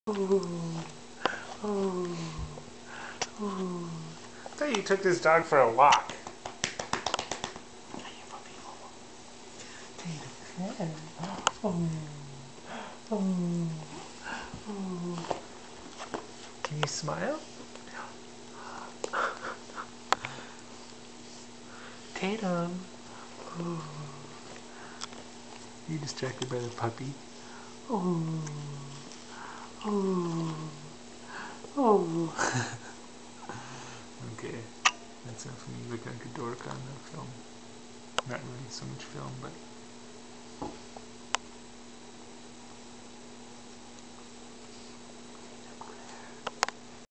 Daddy trying to coax a smile out of Tatum...
Dan came home early and got some quality Tatum time in.